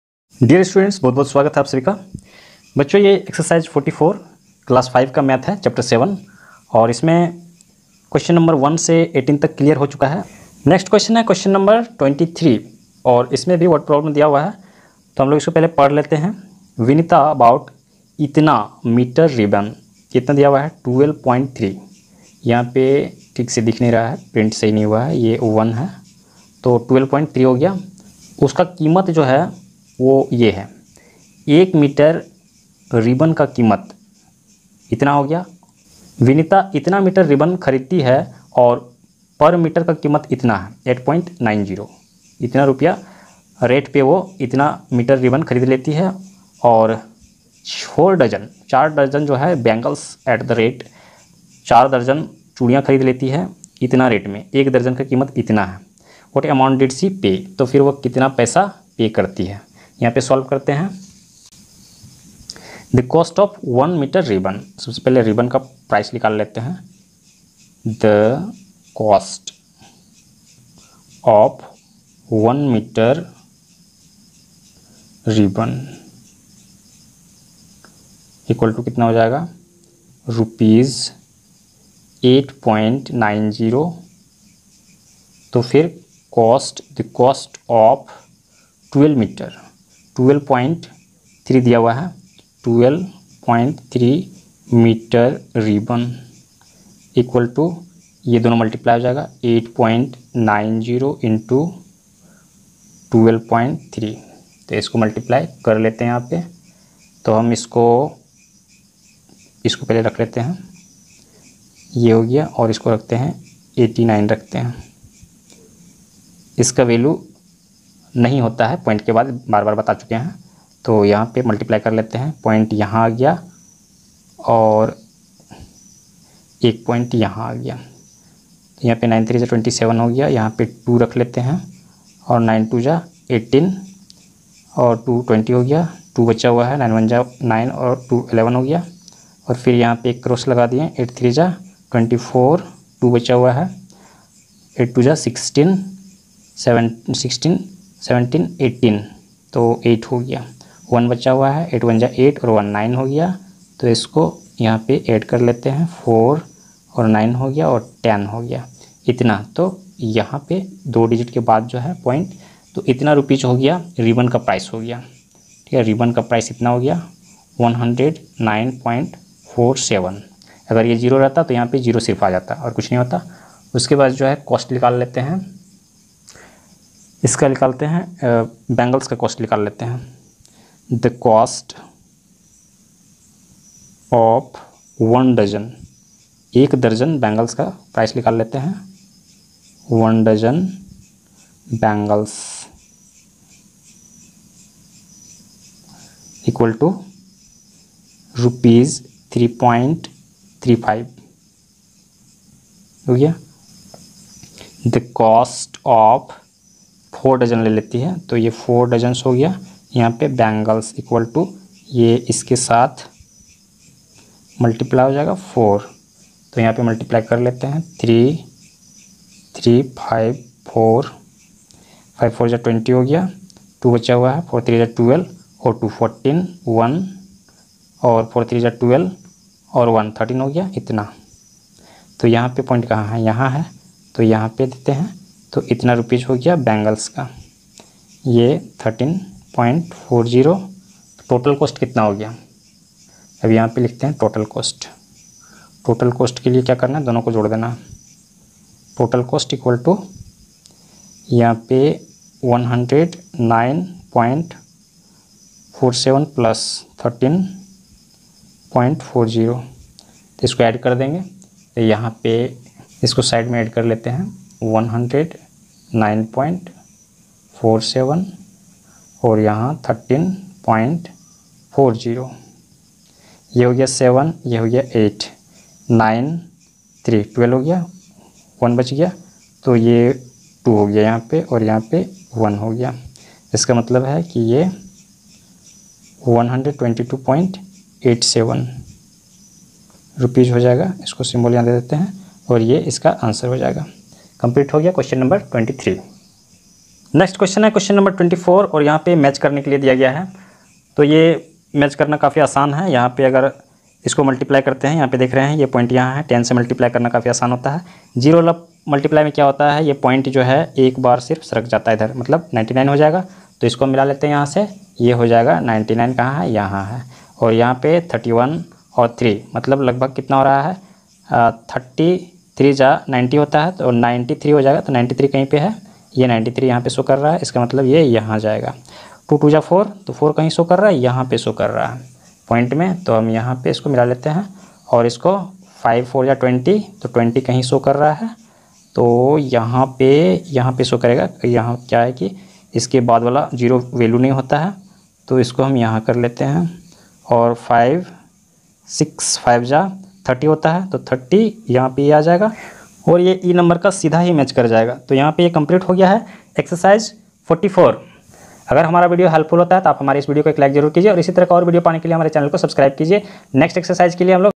Q (23 & 24) Ex 44 Maths Solution Class 5 Ch 7 Saraswati Shishu Vidya Mandir सरस्वती विद्या मंदिर
नमस्कार
बच्चों इस विडियो में Class 5 Maths Ex 44 Q 23 & 24 को Solve किया गया है। आप सभी इस विडियो को ध्यान पूर्वक देखकर समझें और उसे Revise भी करें। धन्यवाद।